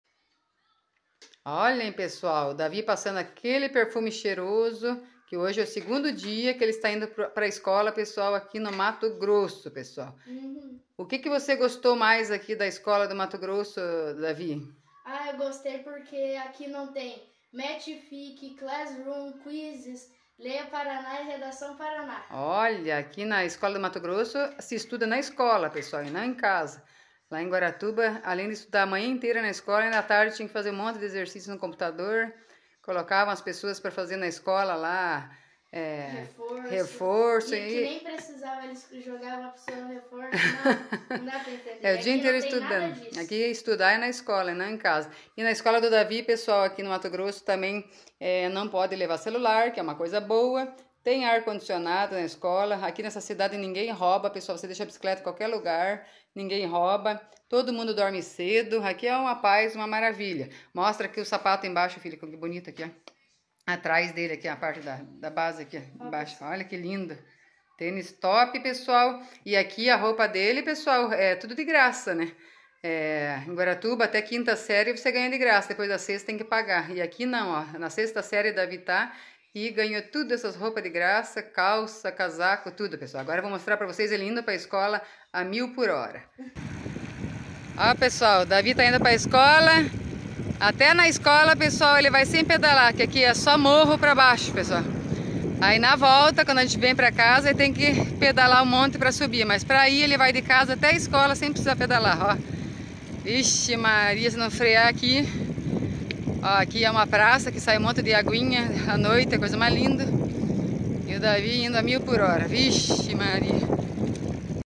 2⁰ dia de aula do Davi Hoinacki aqui no Mato Grosso. 😍🎒🏫✂️
2⁰ dia de aula do Davi Hoinacki aqui no Mato Grosso. 😍🎒🏫✂️